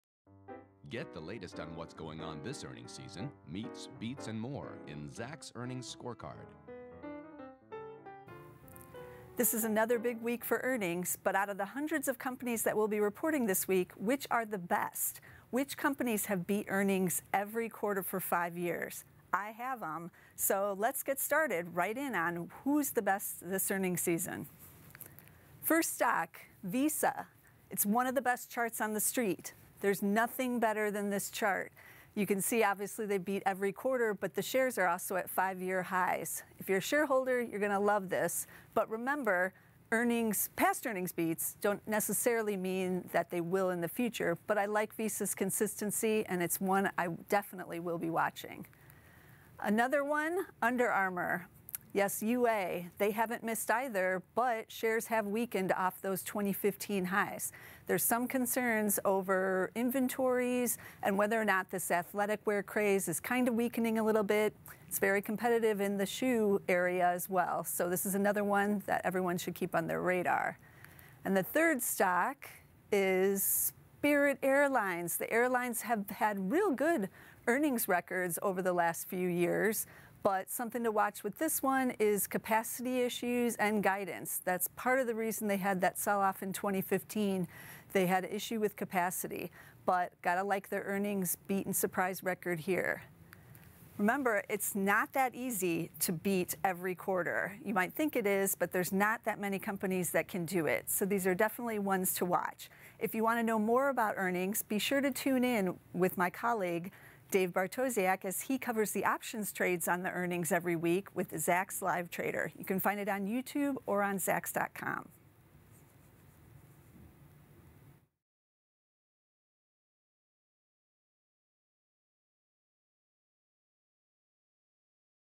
The Best Earnings Charts on the Street
Tracey finds the companies with the best earnings surprise track records this week.